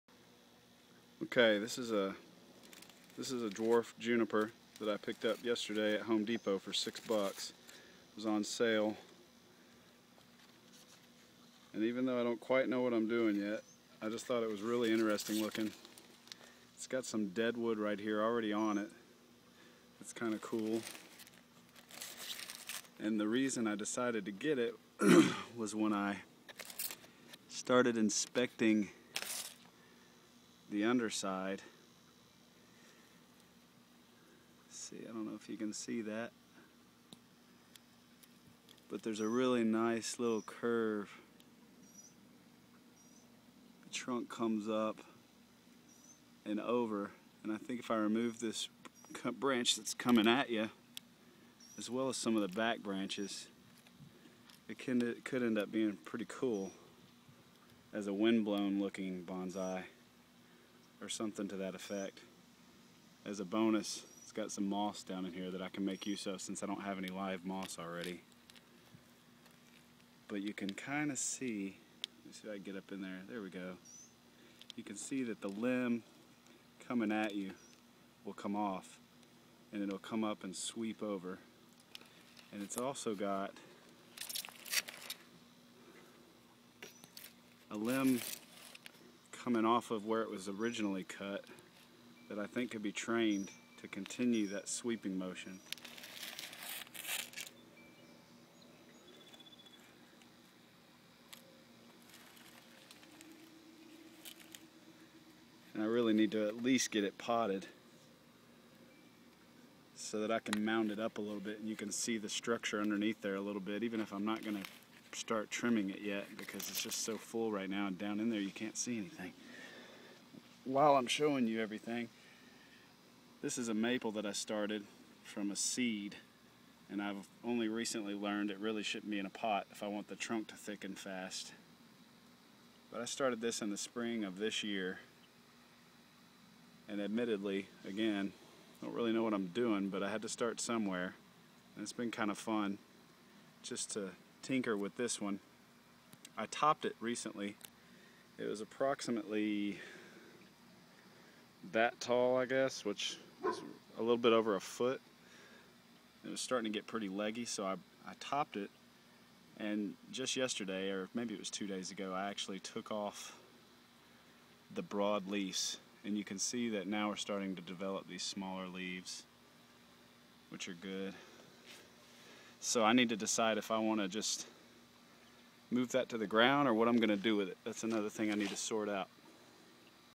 Bonsai noob
This is a video of two of the three bonsai 'experiments' I have going on now